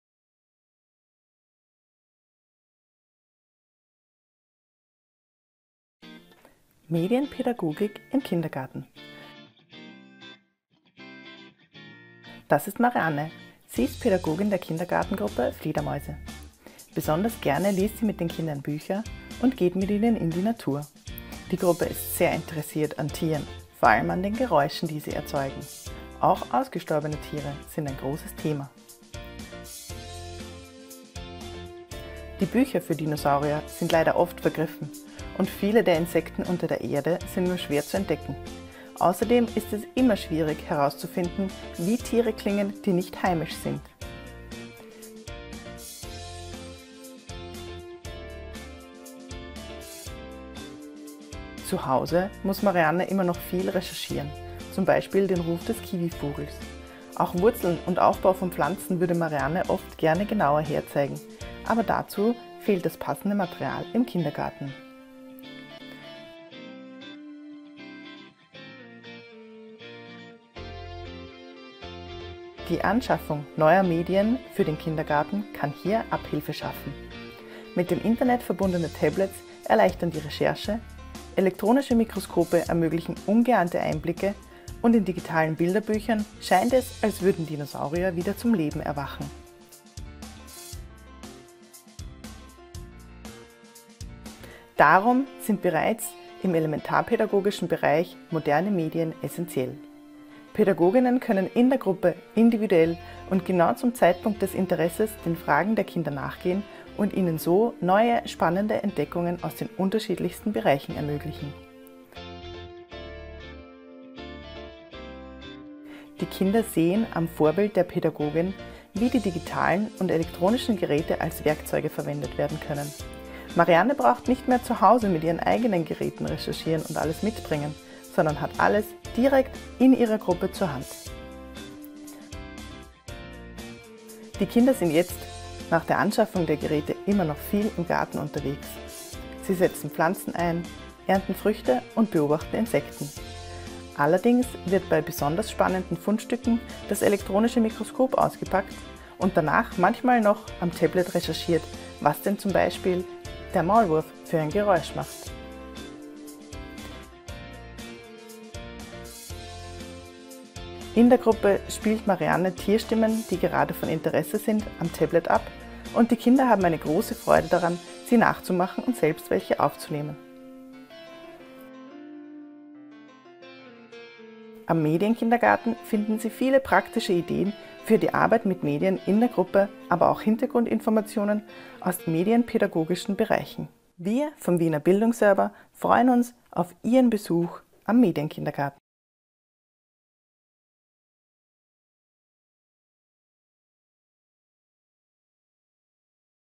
Medienpädagogik im Kindergarten
In diesem Video erfahren Sie warum Medienpädagogik schon im Elementrabereich sinnvoll und notwendig ist.